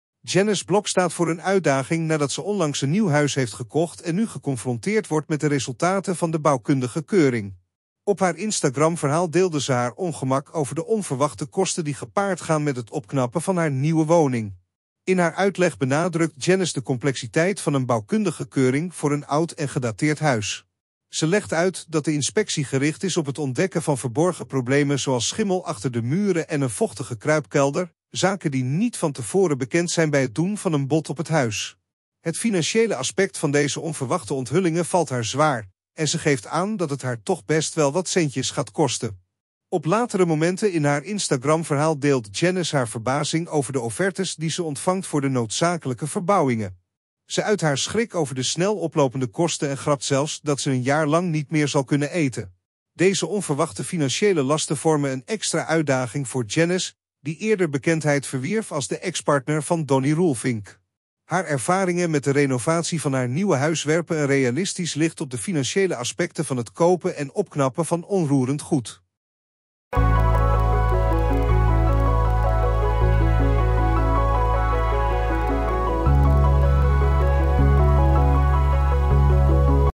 Gigantische tegenvaller voor Janice Blok!
Janice Blok staat voor een uitdaging nadat ze onlangs een nieuw huis heeft gekocht en nu geconfronteerd wordt met de resultaten van de bouwkundige keuring. Op haar Instagram-verhaal deelde ze haar ongemak over de onverwachte kosten die gepaard gaan met het opknappen van haar nieuwe woning.
In haar uitleg benadrukt Janice de complexiteit van een bouwkundige keuring voor een oud en gedateerd huis. Ze legt uit dat de inspectie gericht is op het ontdekken van verborgen problemen zoals schimmel achter de muren en een vochtige kruipkelder, zaken die niet van tevoren bekend zijn bij het doen van een bod op het huis. Het financiële aspect van deze onverwachte onthullingen valt haar zwaar, en ze geeft aan dat het haar 'toch best wel wat centjes gaat kosten'.
Op latere momenten in haar Instagram-verhaal deelt Janice haar verbazing over de offertes die ze ontvangt voor de noodzakelijke verbouwingen. Ze uit haar schrik over de snel oplopende kosten en grapt zelfs dat ze een jaar lang niet meer zal kunnen eten. Deze onverwachte financiële lasten vormen een extra uitdaging voor Janice, die eerder bekendheid verwierf als de ex-partner van Donny Roelvink. Haar ervaringen met de renovatie van haar nieuwe huis werpen een realistisch licht op de financiële aspecten van het kopen en opknappen van onroerend goed.